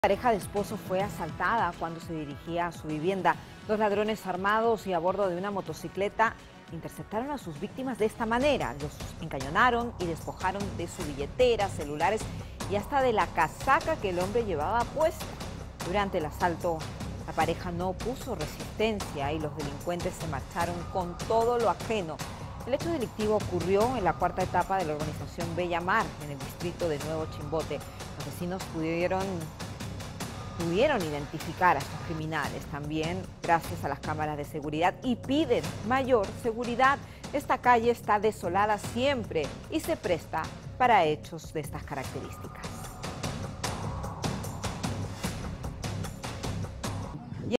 Chimbote: esposos son asaltados y se llevan hasta la casaca que el hombre llevaba puesta
En Chimbote, una pareja de esposos fue asaltada cuando se dirigía a su vivienda caminando tranquilamente. Durante el asalto la pareja no opuso resistencia y los delincuentes se marcharon con todo lo ajeno.

Dos ladrones armados y a bordo de una motocicleta interceptaron a sus víctimas, cuando cruzaban una amplia pista que, a esa hora, aparentemente, no tenía presencia de autos ni transeúntes.

Uno de los sujetos se bajó de la moto y empezó a rebuscar entre la ropa de sus víctimas cualquier objeto de valor mientras los encañonaban. Los despojaron de billeteras, celulares y hasta la casaca que el hombre llevaba puesta.

NECESITAN MÁS RESGUARDO POLICIAL

El hecho delictivo ocurrió en la cuarta etapa de la urbanización Bellamar, en el distrito de Nuevo Chimbote. Los vecinos pidieron mayor seguridad ya que esta calle es desolada y siempre ocurren hechos como este.